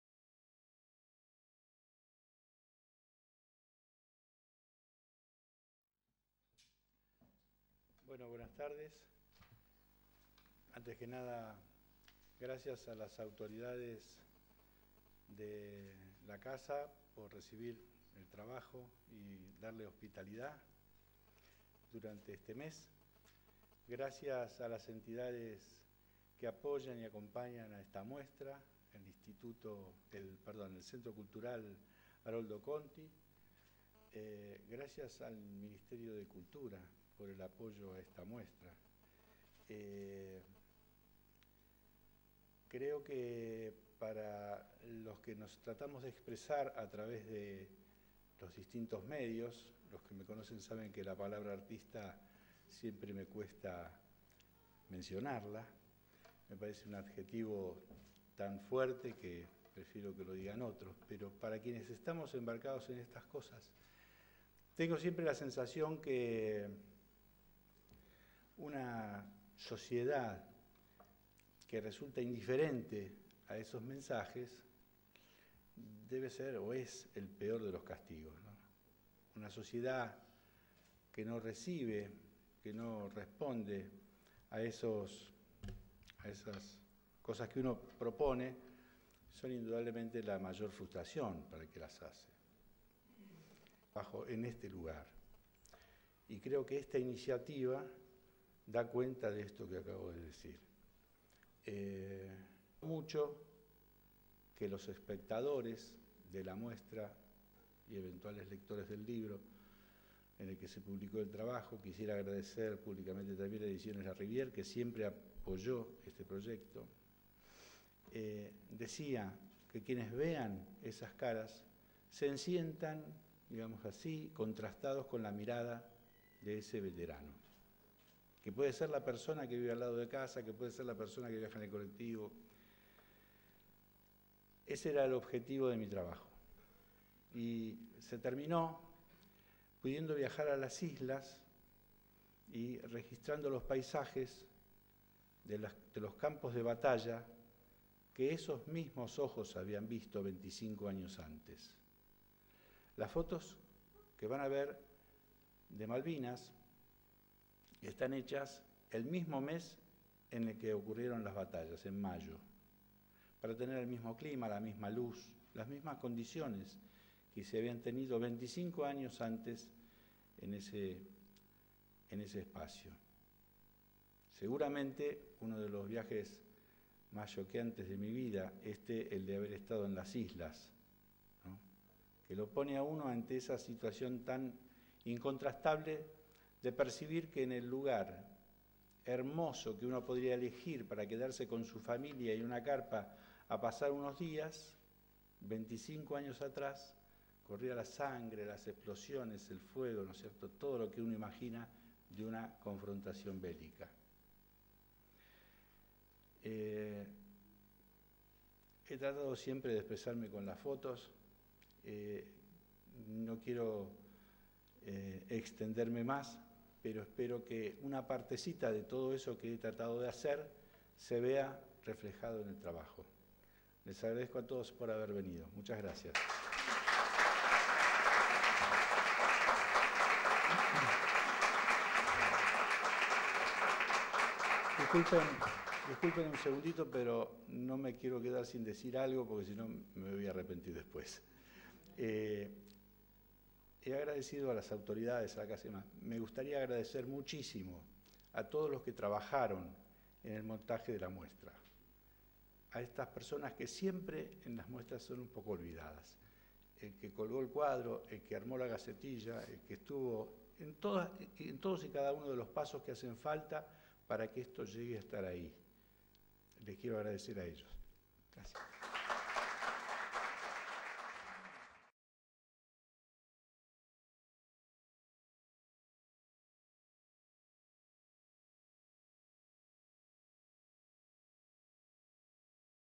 Muestra "Malvinas. Retratos y paisajes de guerra" - Juan Travnik - 06/07/2015
Se inauguró la muestra "Malvinas. Retratos y paisajes de guerra", en el Banco Central de la República Argentina (BCRA), junto al Ministerio de Cultura de la Nación, el Centro Cultural de la Memoria Haroldo Conti y el apoyo institucional del Fondo Nacional de las Artes.

La muestra está conformada por 63 retratos blanco y negro realizados por el fotógrafo argentino Juan Travnik en distintas ciudades del país, y plasma el rostro de ex-combatientes de Malvinas entre 1994 y 2007. Completan la exhibición 12 fotografías de paisajes tomadas en las Islas Malvinas en mayo de 2007, que ponen a la vista los escenarios en los que se desarrolló el conflicto: campos de batalla, refugios, restos de armamentos, aviones, helicópteros destruidos, entre otros.

Además de la muestra fotográfica, se expondrán monedas y billetes históricos y actuales y otros elementos pertenecientes a la exhibición “Malvinas. Historia y presente” del Museo del Banco Central que comprende un resumen de los acontecimientos que tuvieron a las Islas como protagonistas.